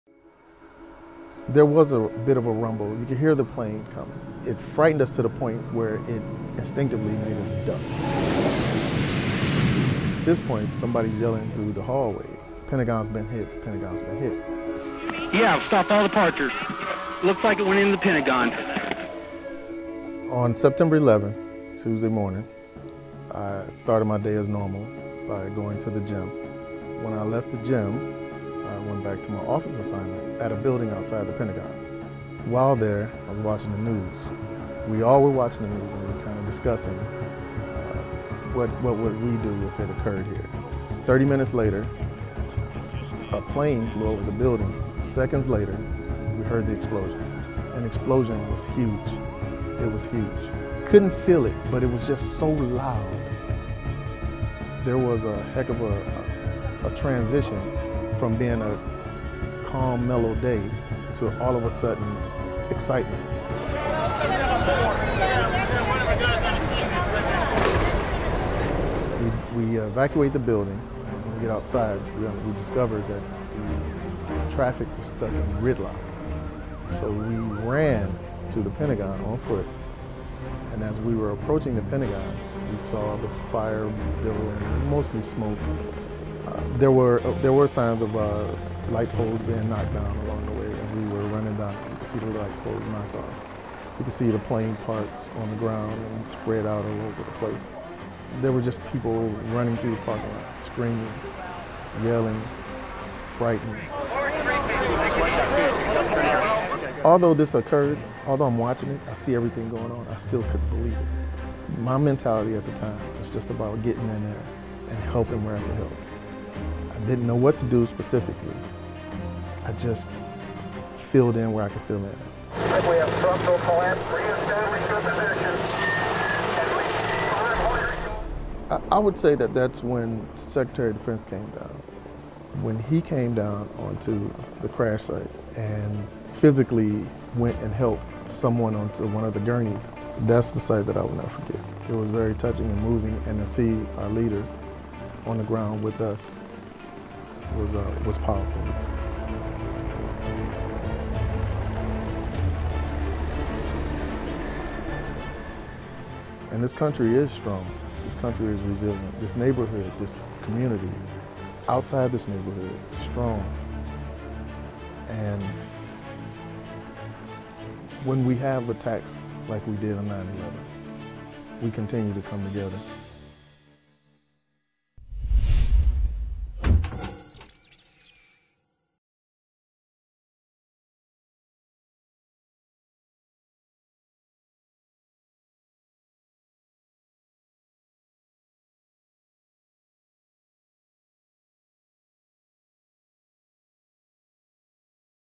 Witness Recounts Attack on Pentagon
A Pentagon Force Protection Agency officer describes the moments following the Sept. 11 attack on the Pentagon, as he and others tried to assist in the aftermath.